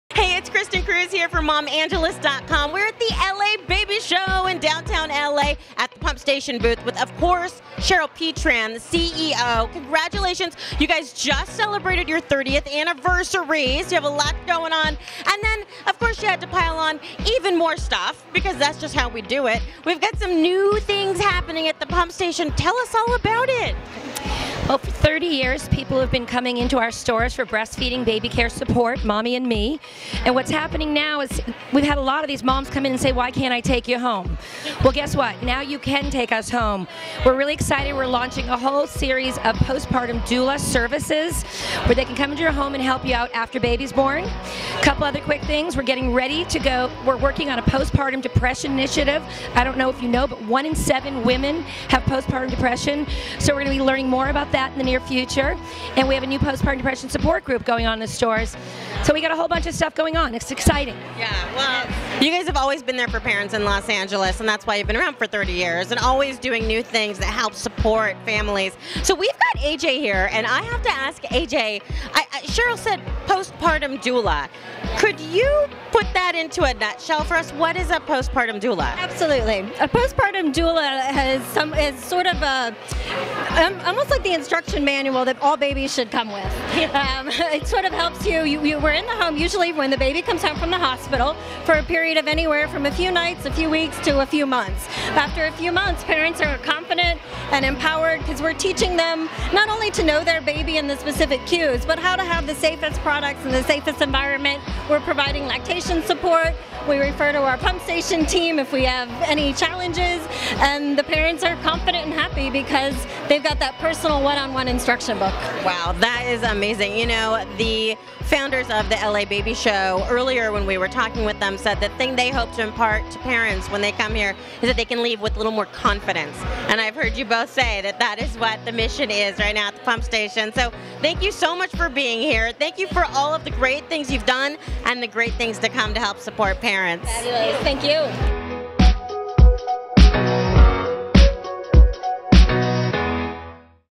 Cheryl Petran and AJ Jonesco
Cheryl Petran and AJ Jonesco of the Pump Station share their latest news with Kristin Cruz of MomAngeles about launching a post partum doula service, celebrating 30 years in business and being honored by the LA Baby Show.